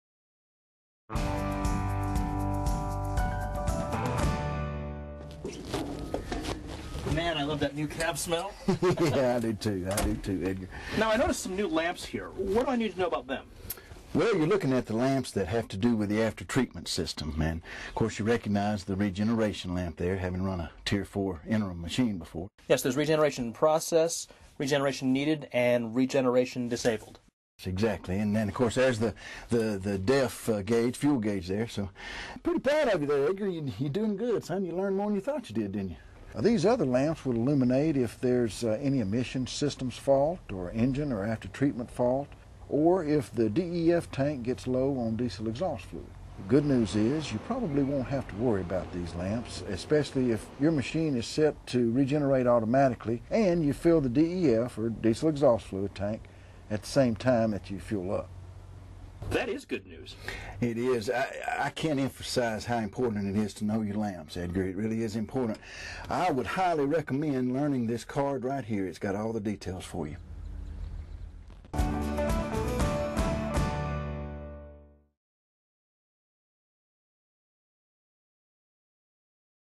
Cat® Tier 4 Final Operator Overview | Know Your Lamps
Overview of the operator interface for Cat® Tier 4 Final products.  Please refer to your specific machine's Operation & Maintenance Manual for more information.